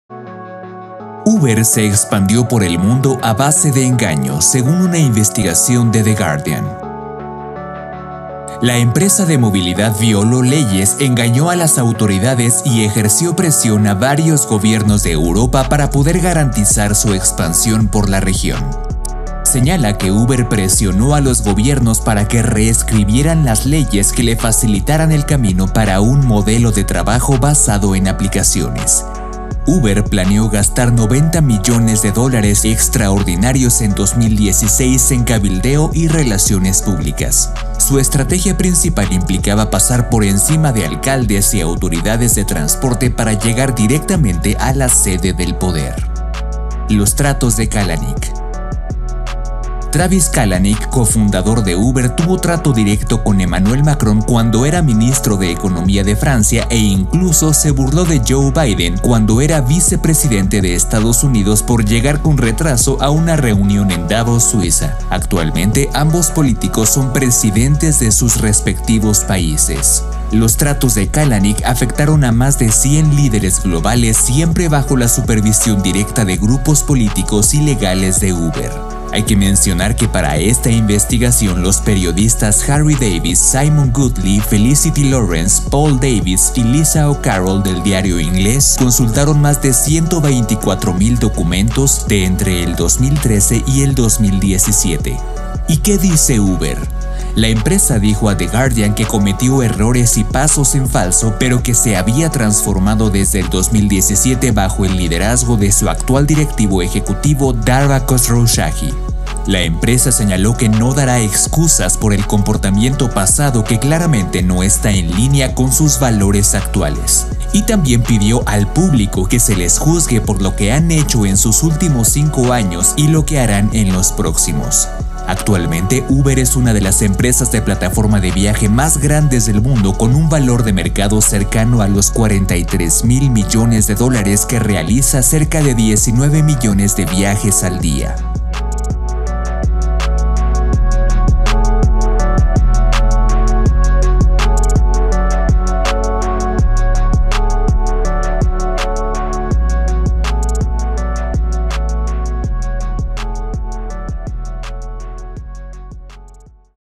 Con ENGAÑOS UBER LOGRÓ EXPANDIRSE por el MUNDO | ÚLTIMAS NOTICIAS
Según una investigación de 'The Guardian', Uber violó leyes, engañó a las autoridades y presionó a varios gobiernos de Europa para poder garantizar su expansión por el mundo.